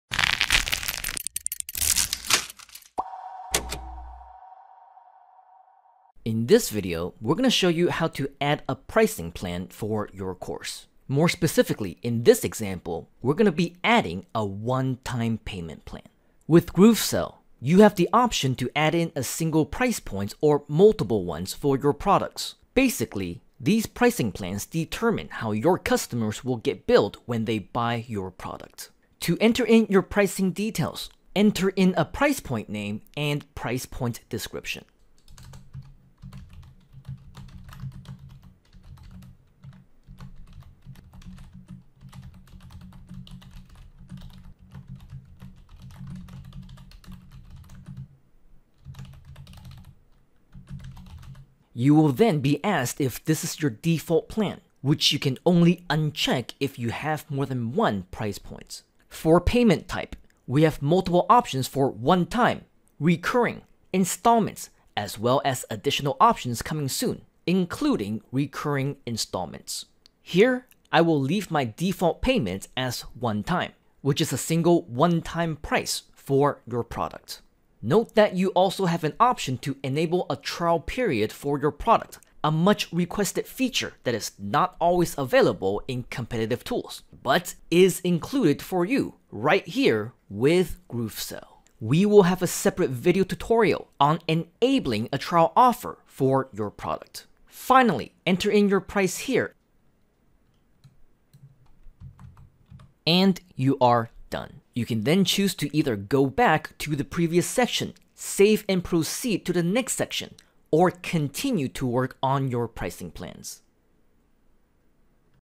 [GrooveFunnels WalkThrough] GrooveSell: Products (Pricing - Add A One-Time Payment Plan)
You have the option to add in a single price point or multiple ones for your products.

Basically, these pricing plans determine how your customers will get billed when they buy your product.

To enter in the details of this price point, enter in a Price Point Name and Price Point Description.

You will then be asked if this is your default plan, which you can only uncheck if you have more than one price point.

For payment type, we have multiple options for One-Time, Recurring, Installments, as well as additional options coming soon, including Recurring Installments.

Here, I will leave my default payment as One-Time, which is a single one-time price for the product.

Note that you also have an option to enable a trial period for your product, a much-requested feature that is not always available in competitive tools, but is included for you right here with GrooveSell.

We will have a separate video tutorial on enabling a trial offer for your product.

Finally, enter in your price here, and you are done.

You can then choose to go Back to the previous section, Save and proceed to the next section, or continue to work on your pricing plans.